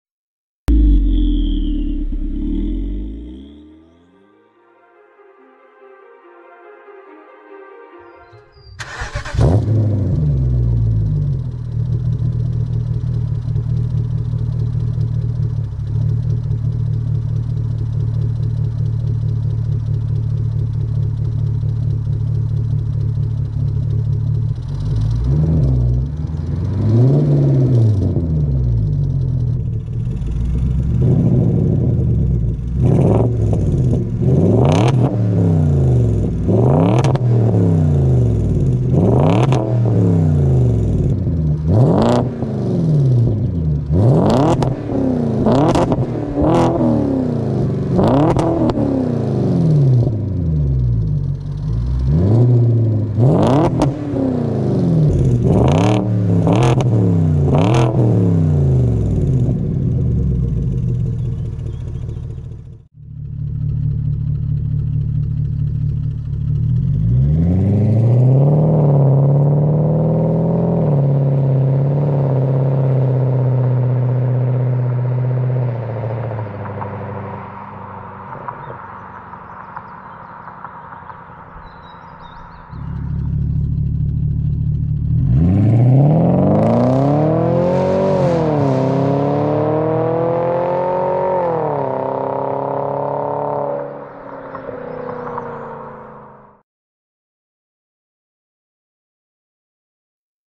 BMW E63 4.4 V8 EXHAUST SOUND | BRZMIENIE UKŁADU WYDECHOWEGO
✅ BMW E63 4.4 V8 w której wykonaliśmy przeróbkę końcowych sekcji układu wydechowego, efekt finalny oceńcie sami! 🤙